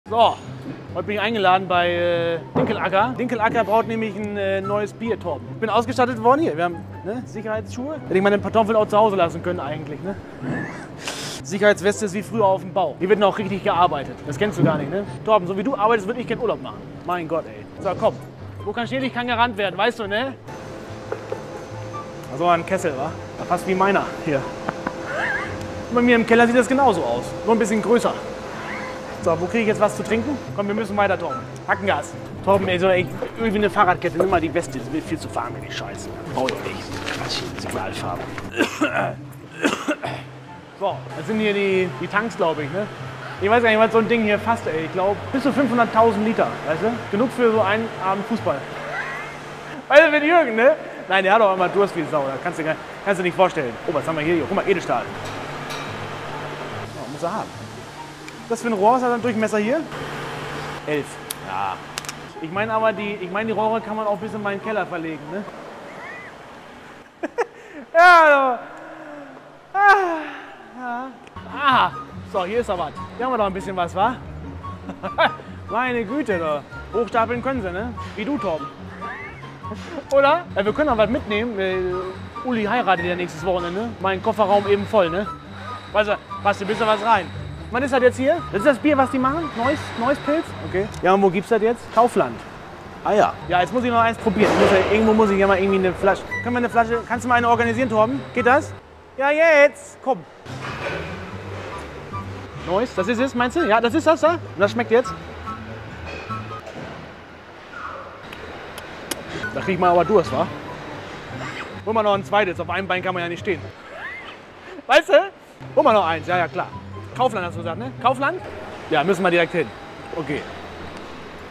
Väter - In Brauereien // Das NOICE Bier ab 14.09. bei Kaufland!
Ab 14.09. gibt es das NOICE Bier deutschlandweit in jedem Kaufland!

Auch der Vadda muss das neue Bier probieren! Dabei wird aber natürlich erst die Anlage auf Herz und Nieren geprüft. Ob das Bier etwas kann? Seht selbst im Video!